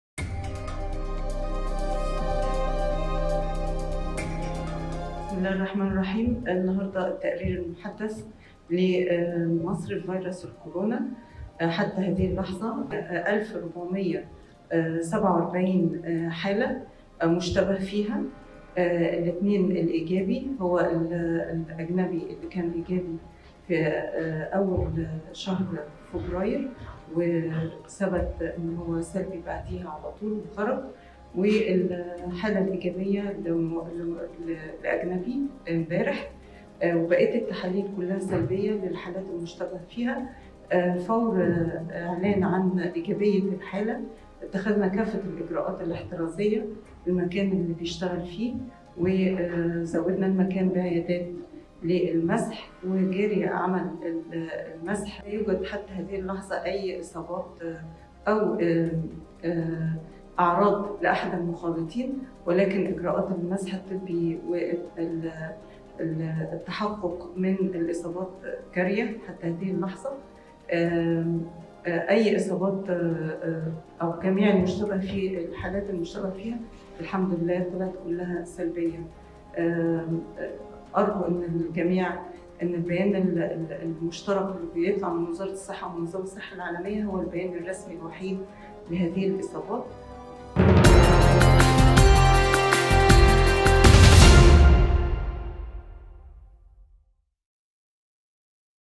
من الصين.. وزيرة الصحة المصرية تكشف تفاصيل إصابات فيروس كورونا
العين الإخبارية | من الصين.. وزيرة الصحة المصرية تكشف تفاصيل إصابات فيروس كورونا
أعلنت هالة زايد، وزيرة الصحة المصرية، عن إجراء تحاليل لـ 1447 حالة مشتبه فى إصابتها بفيروس كورونا المستجد (كوفيد-19)، مؤكدة أن جميعها جاءت سلبية ما عدا حالتى الشخصين الأجنبيين والذين تم الإعلان عنهما مسبقًا